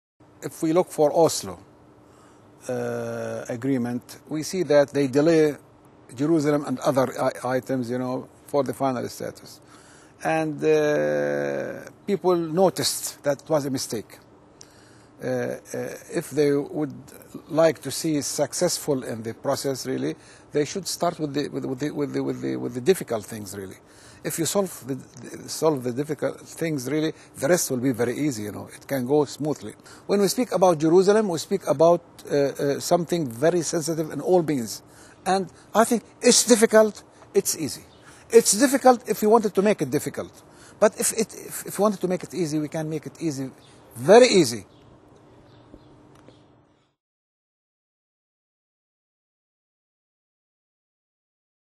'Abbas' Adviser: Jerusalem First
'Adnan Hussieni says it is time to discuss the core issues behind the Palestinian- Israeli conflict.